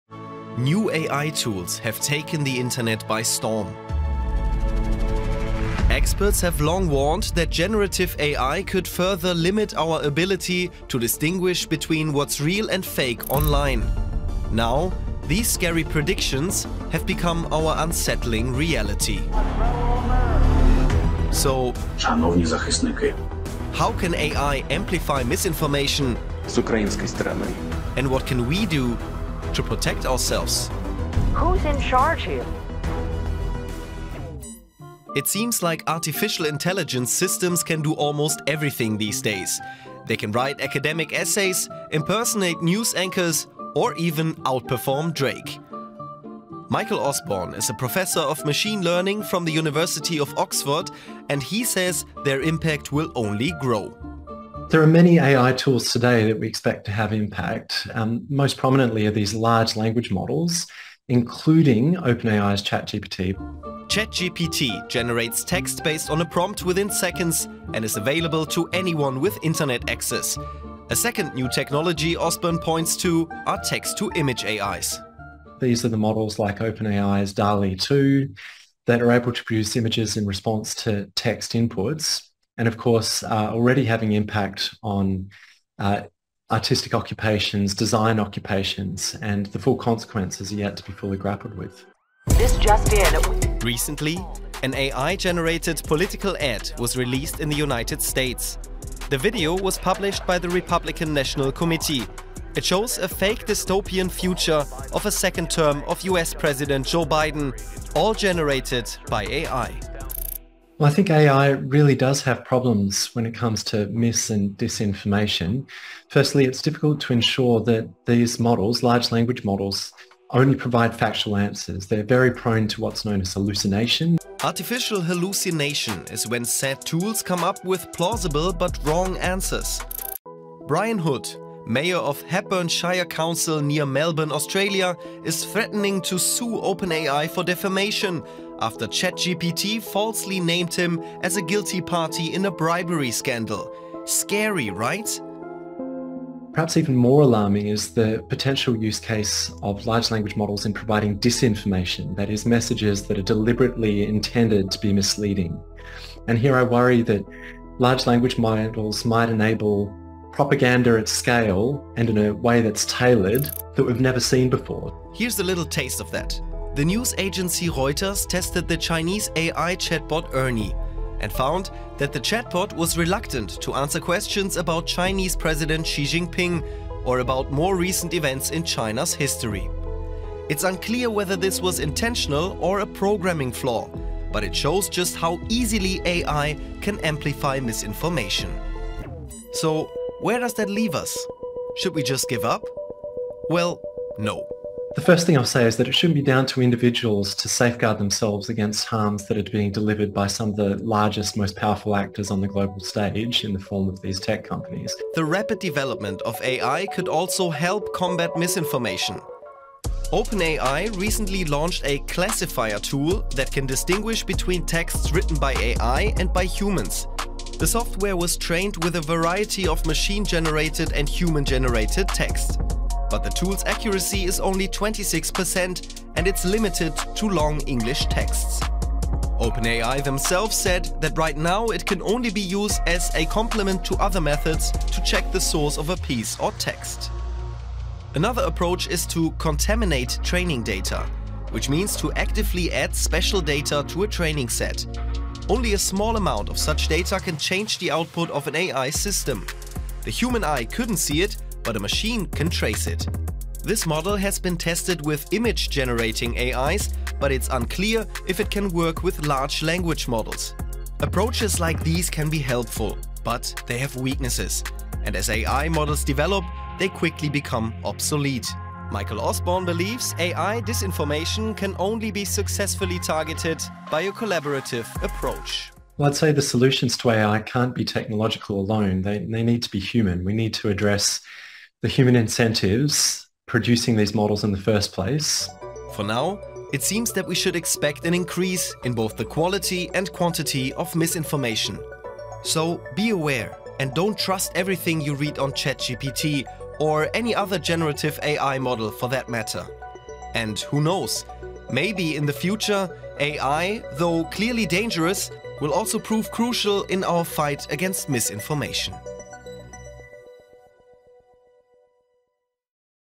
Generative AI spreading Fake News
AI tools can be fun. But once their filters are breached, they can amplify misinformation - and they are doing it already. We look at what this can entail, and how we can fight back!